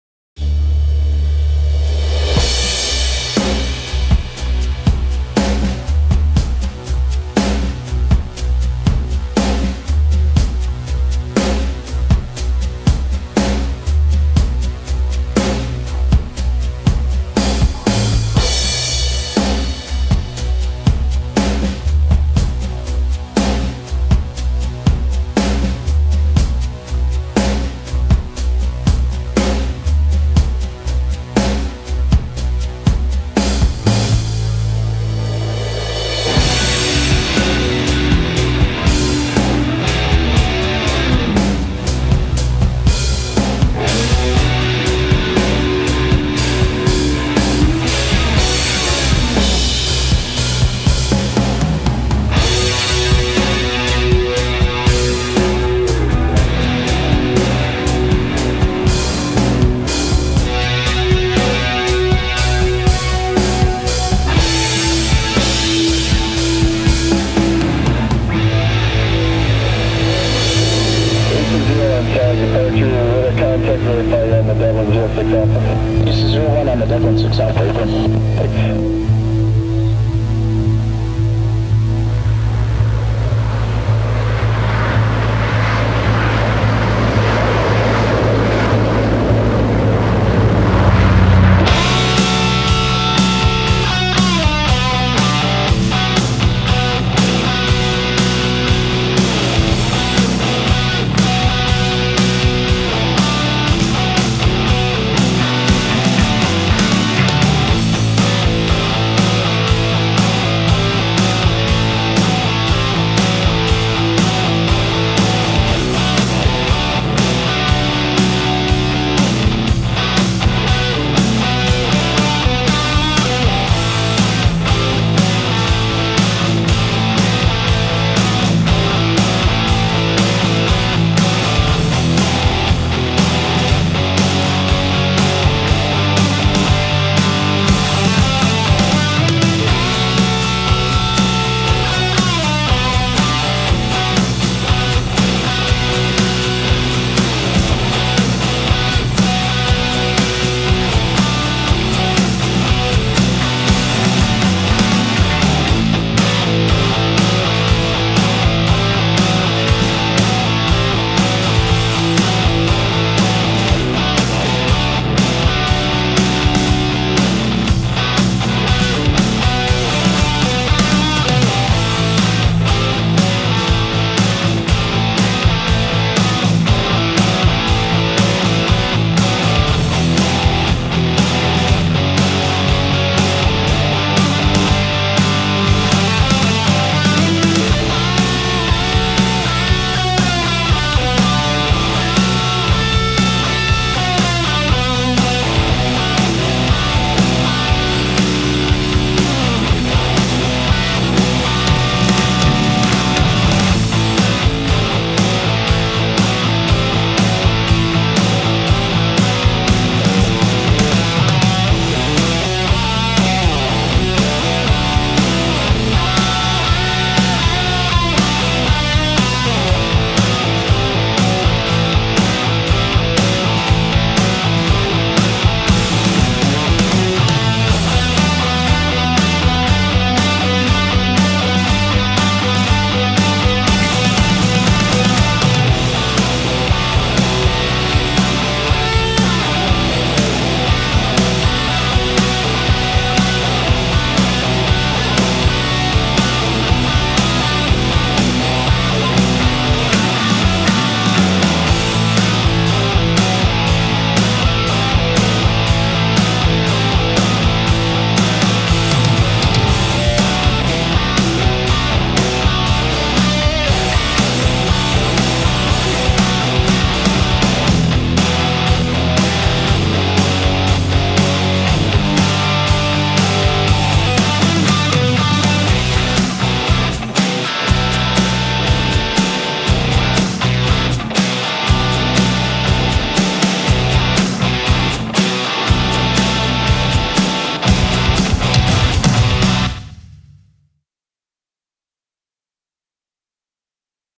US Navy Carrier Force || Active Operations: Music Tribute
A music tribute to Naval war games and training operations.